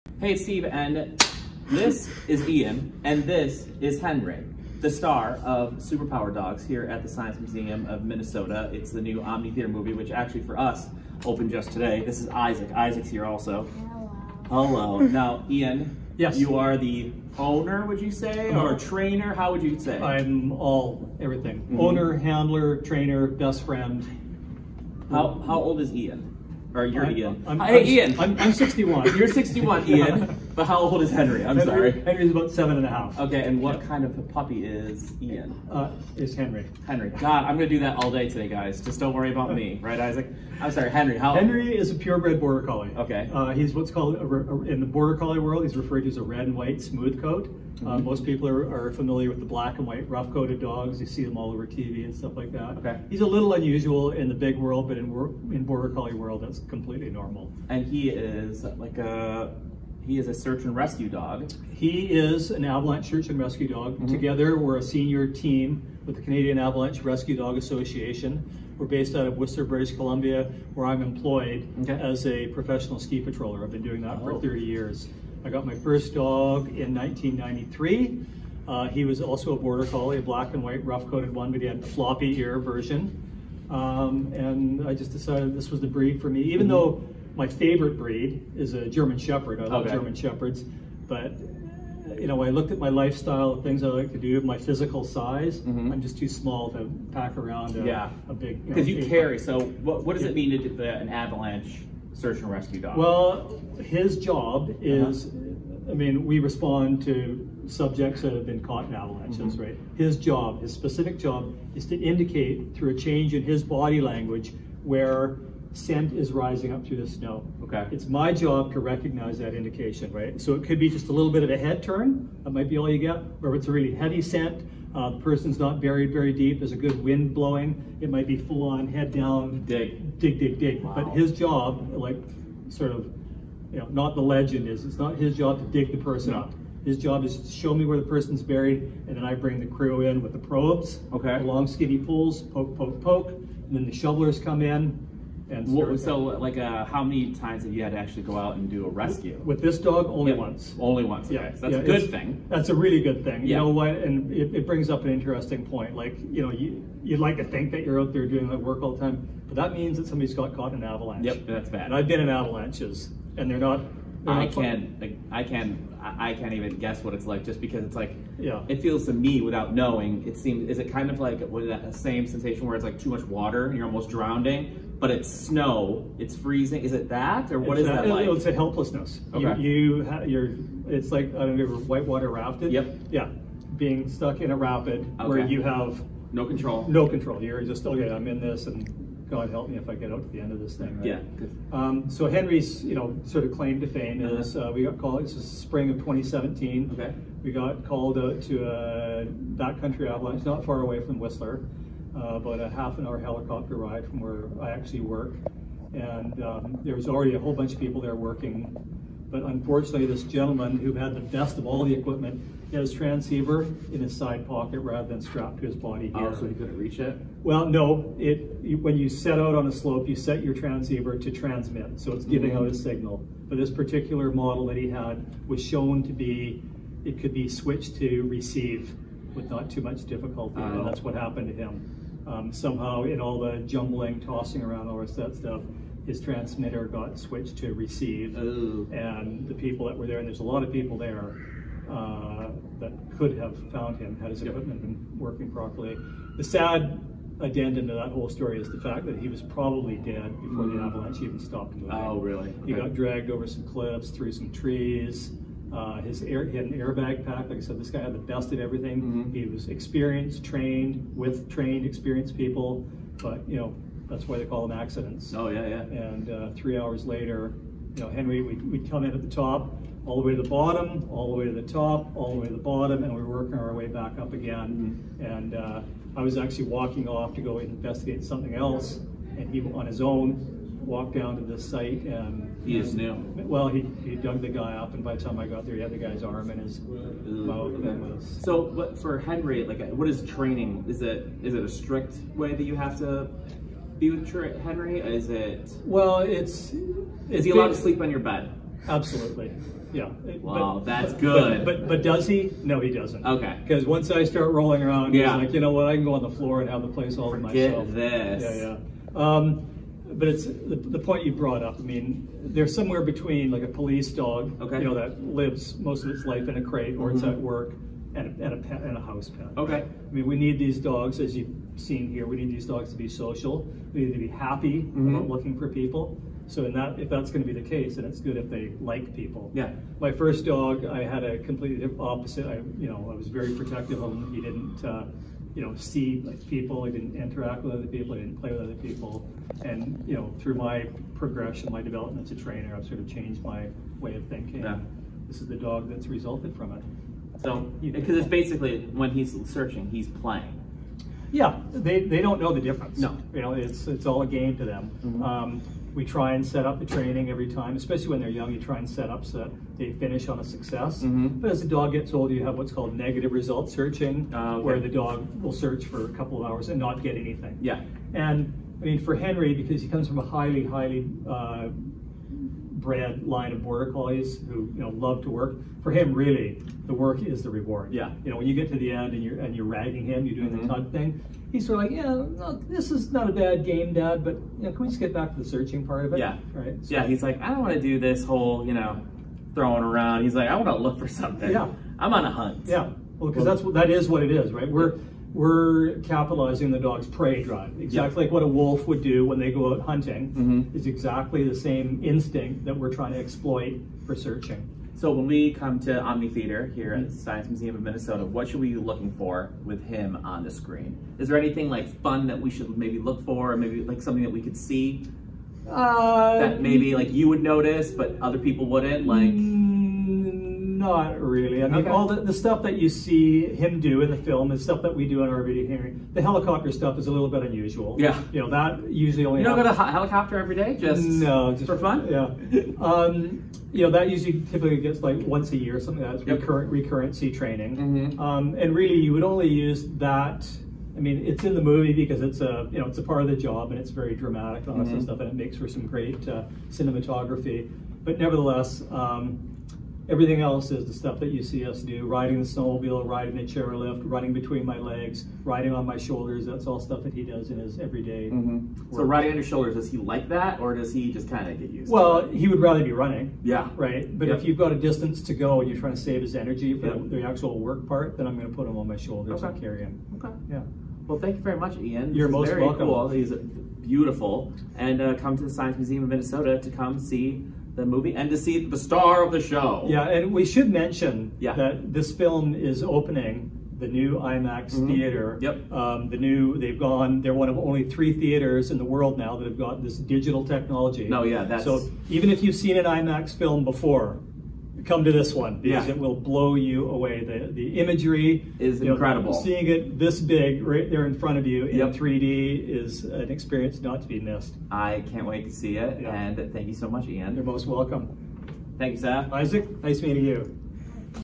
Henry and Ian from from Super Power Dogs
Steve-O sits down to talk with Ian about Henry, the star of Super Power dogs the new film showing now at the Omni Theater at the Science Museum of Minnesota.